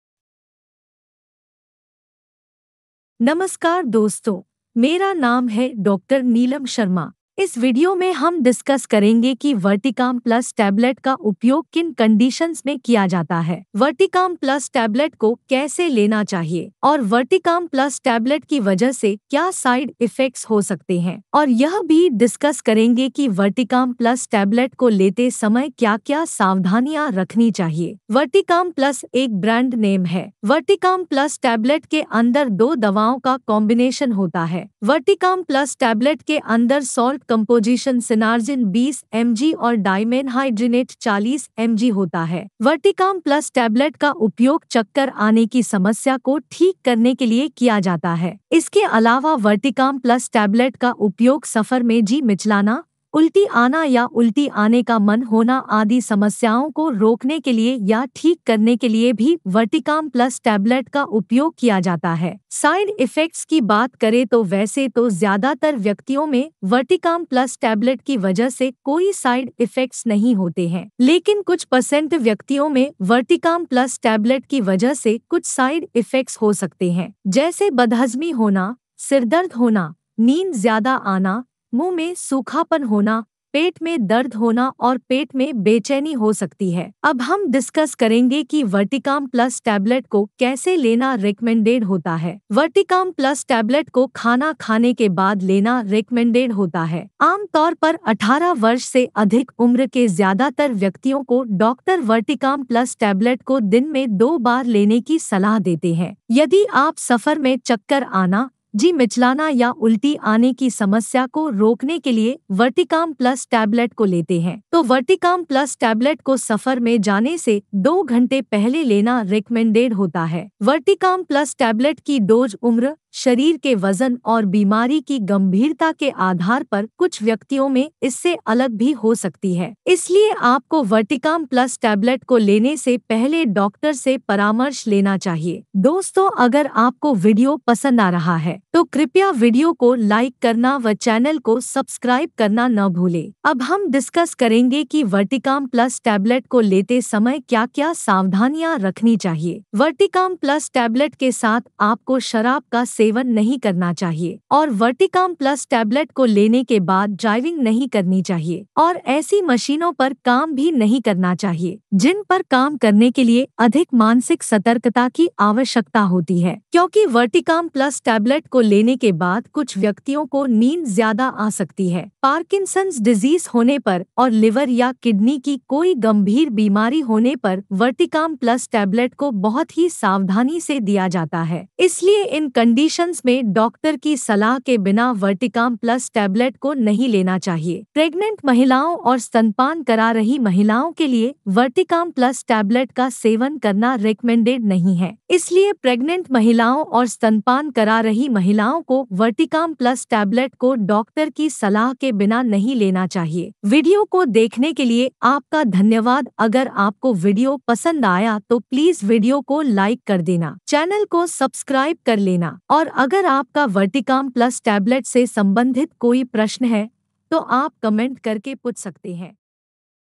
Verticalm Plus Tablet Uses & Side Effects in Hindi
in this video, we will discuss what are the uses and benefits of verticalm plus tablet. How to use verticalm plus tablet. We will also discuss what are the side effects of verticalm plus tablet. Watch the complete video for detailed review of verticalm plus tablet in hindi.